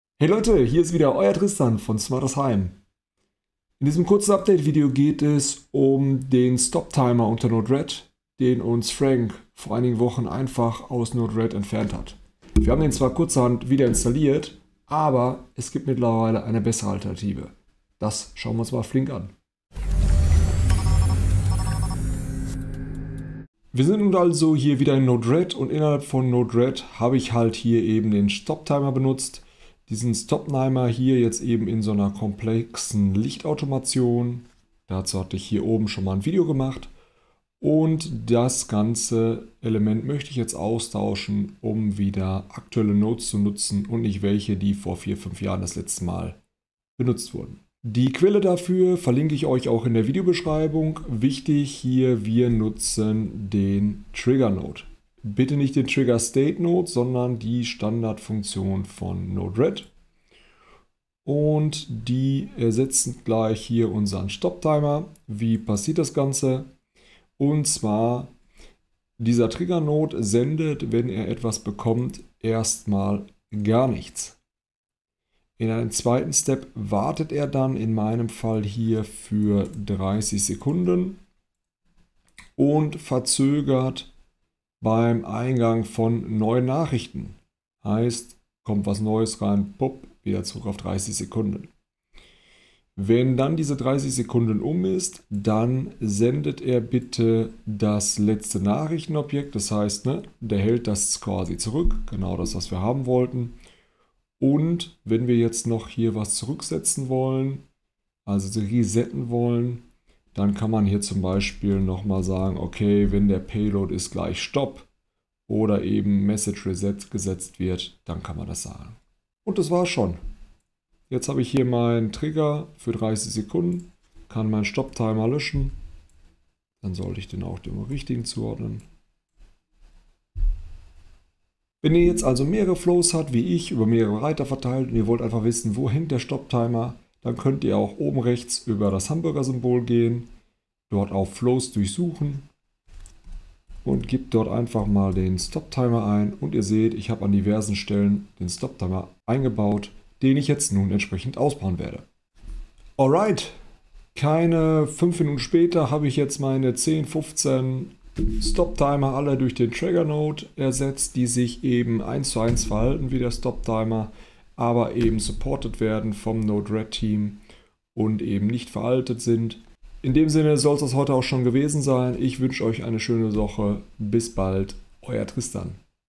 QuickTipp: StopTimer in Node-RED ersetzen
In Node-RED ersetze ich den Stoptimer mit dem Tigger-Node. Die entsprechende Einrichtung zeige ich euch in diesem Video.

Neben dem Inhalt geht es mir in diesem Video vor allem auch um die Technik. Passt der Sound, das Bild? Euer Feedback ist Willkommen!